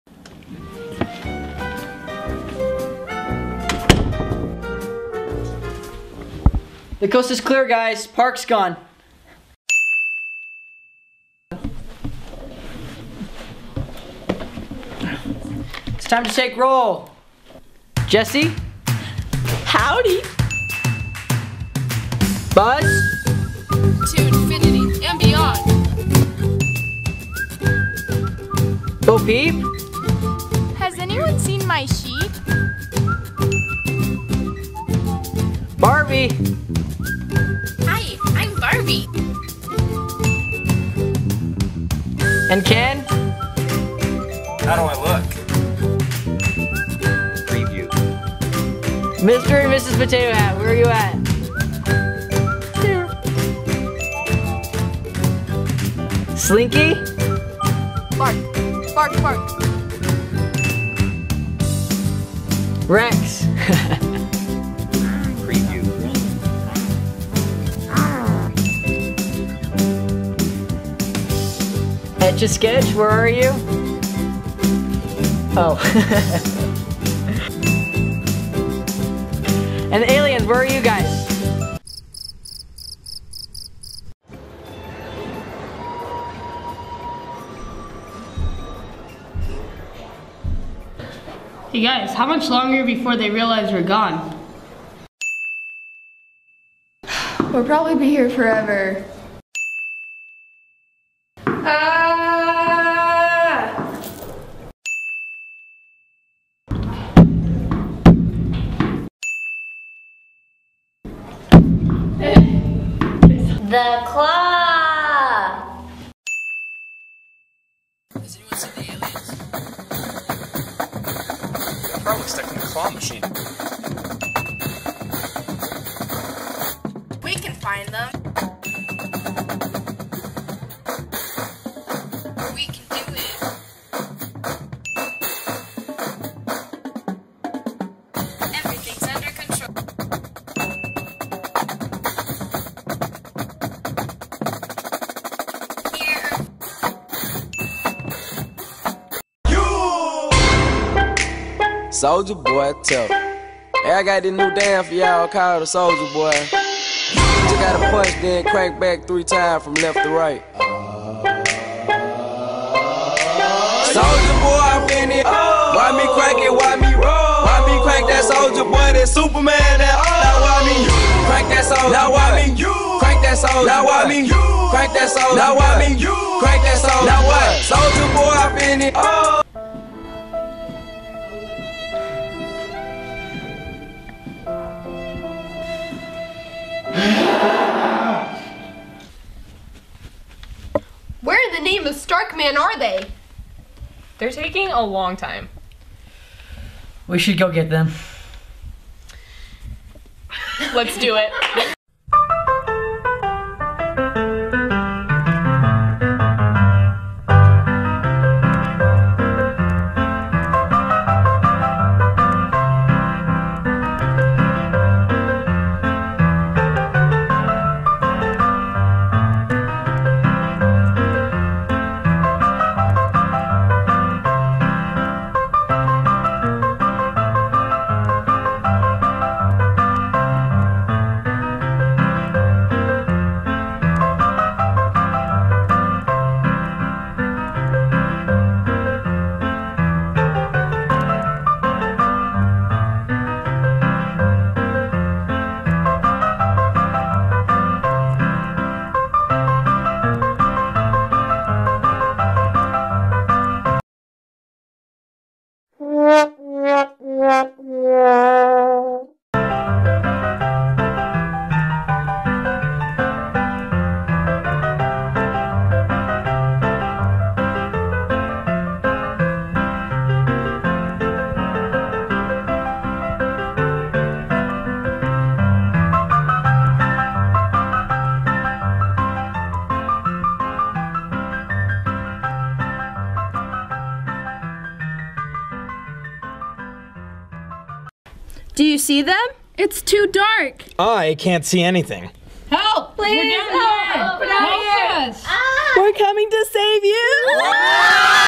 Blue Team Video: Toy Story (2016)
Timpanogos High School Student Council Elections 2016 Blue Team's Final Video. You've got a friend in us.